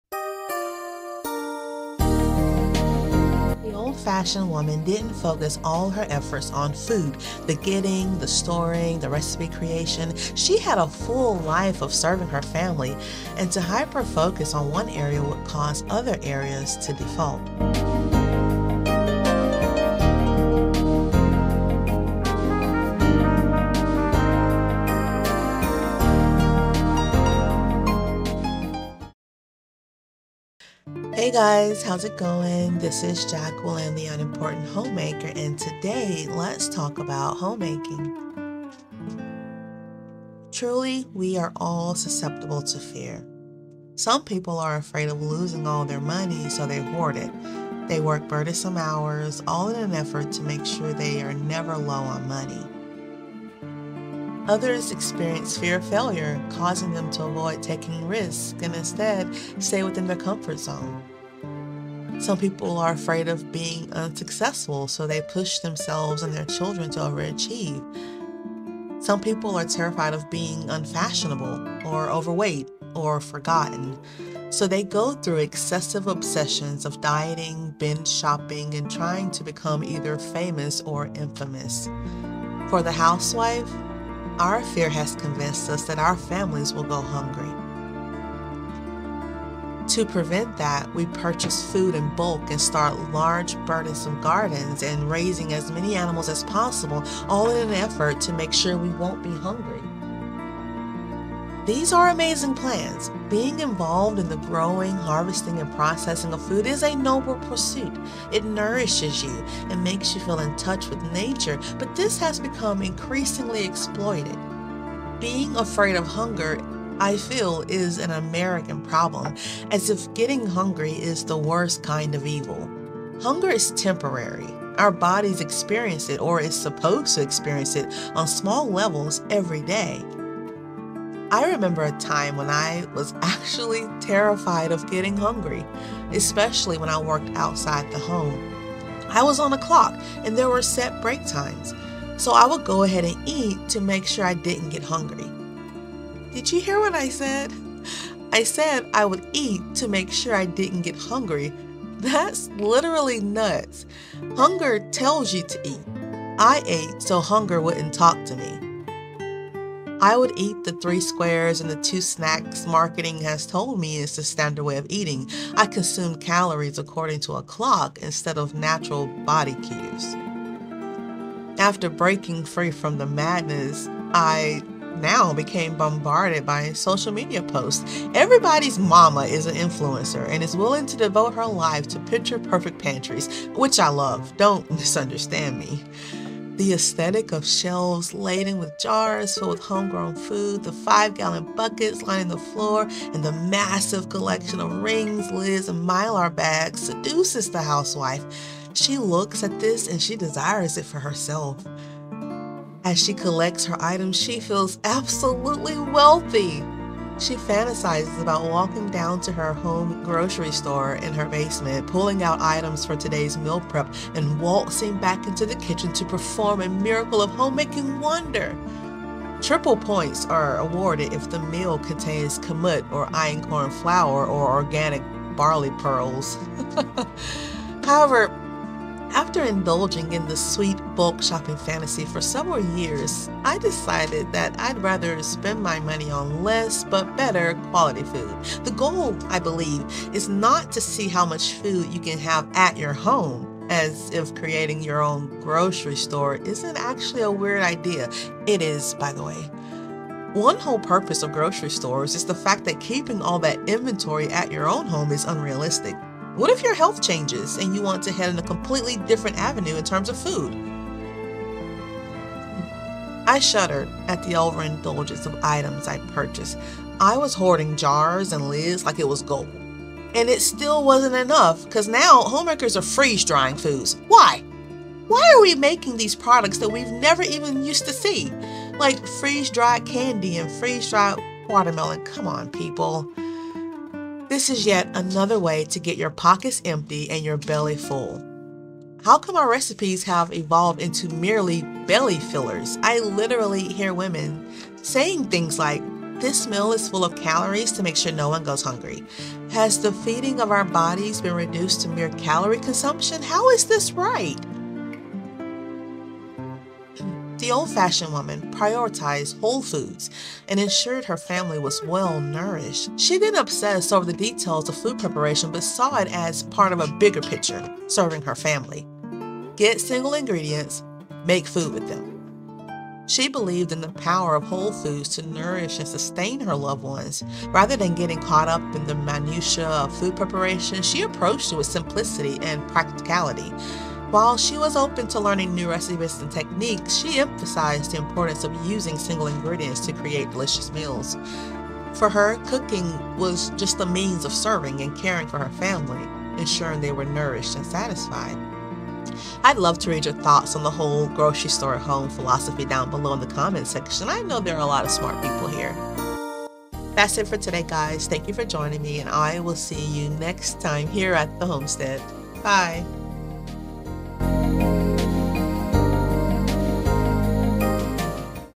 Desperate (and HUNGRY) Housewives
As a Christian homemaker, you have the incredible opportunity and responsibility to create a home that reflects God's love and grace. I want to share with you the power of setting your heart's intention and living out the word of God.

Jacqueline Brown
Nicholson, MS 39463

Amazon Store Front